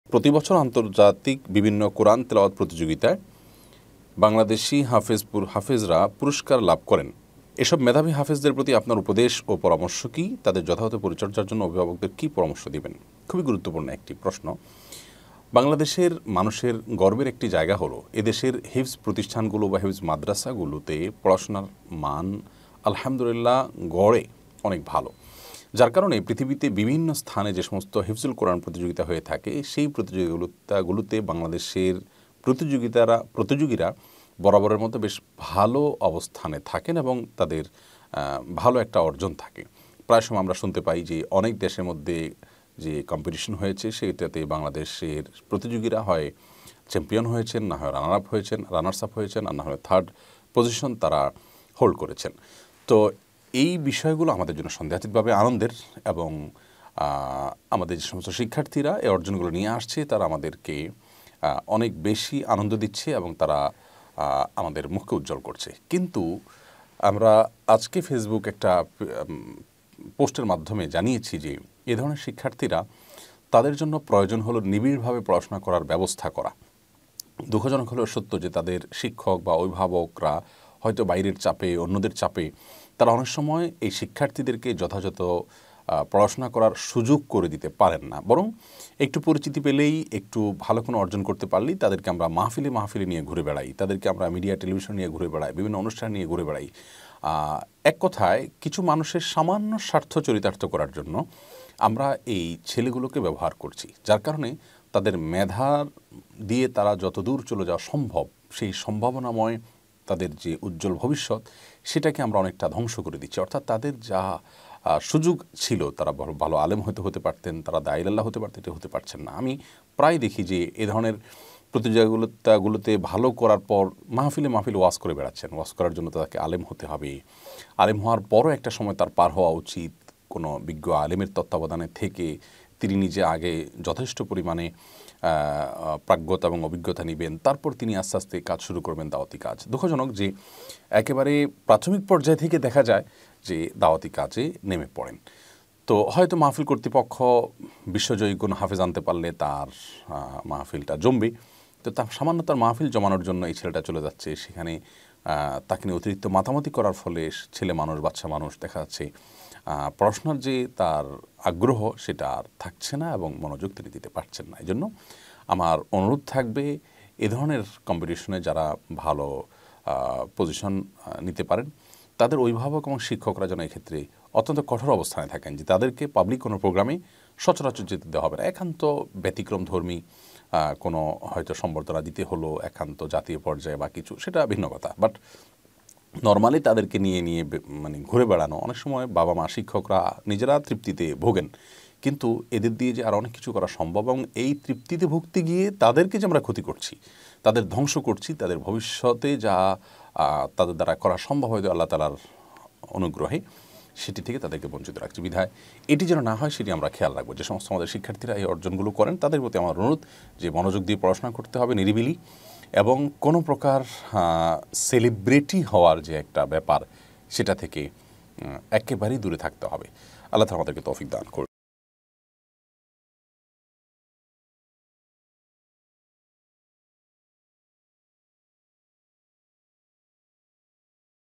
বিশ্বজয়ী হাফেজদেরকে কীভাবে বিশ্বজয়ী আলেম বানাবেন?
দেশে বিশ্বজয়ী হাফেজ হয়, বিশ্বজয়ী আলেম হয় না কেন? অভিভাবকদের প্রতি অনুরোধ

শরয়ী সমাধান II Sheikh ahmadullah live